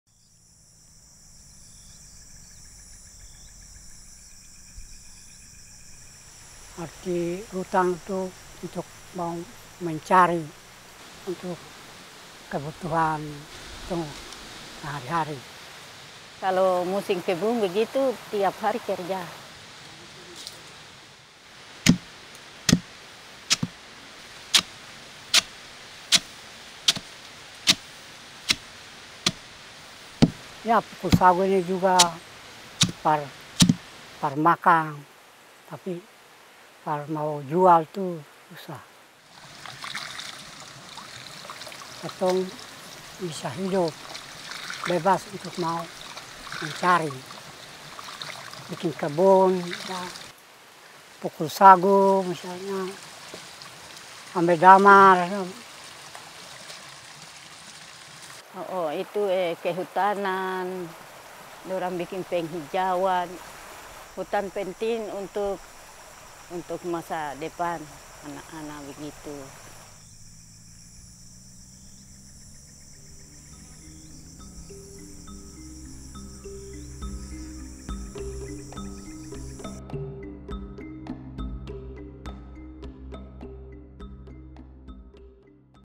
Changing times: Living off the forest in Maluku, Indonesia
Elderly couple Silas Matoke and Yordana Yawate have lived off the forest their entire lives in Maluku, Indonesia. But as laws change and populations grow, it’s becoming harder to meet their daily needs.